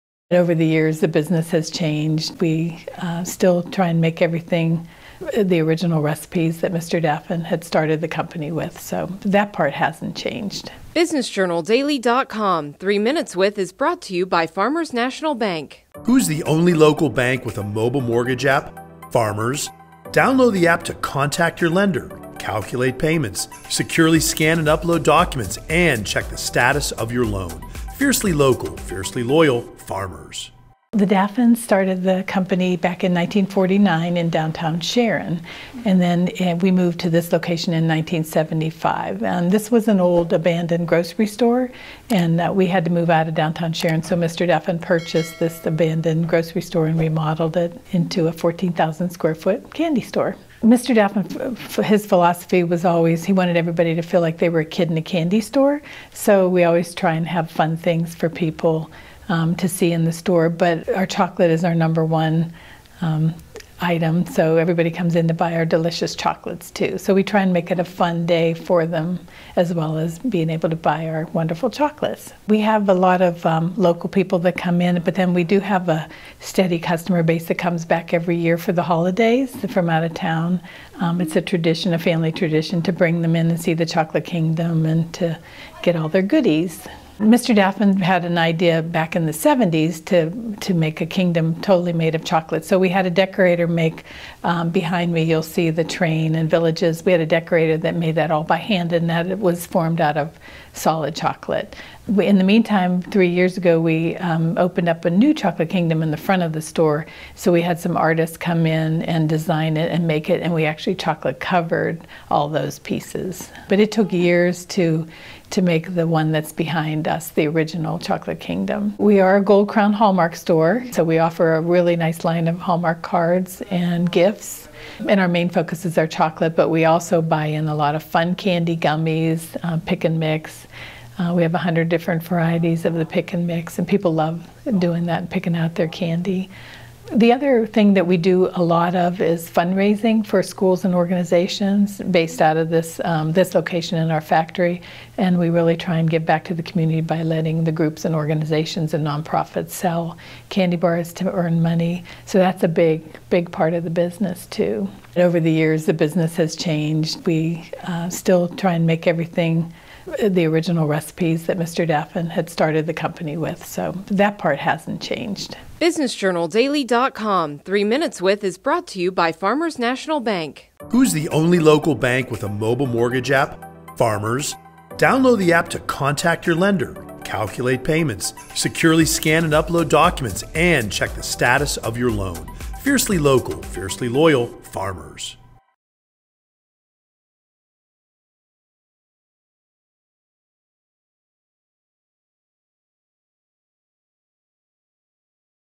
Daffin's Candies Sticks to What Works | 3 Minutes With 9-5-22
SHARON, PA -- In 1949, Paul “Pete” Daffin opened the first Daffin’s Candies store in downtown Sharon, Pa. In 1975, Daffin purchased an abandoned grocery store and remodeled it into a 14,000 square foot candy store.

Connie Leon, retail manager who joined Daffin’s in 1966, says over the years, the business has changed, but they still try and make everything using the original recipes Mr. Daffin started the company with.

“Mr. Daffin’s philosophy was that he always wanted everyone to feel like they were a kid in the candy store,“ says Leon. “We always try to have fun things in the store for people to see.”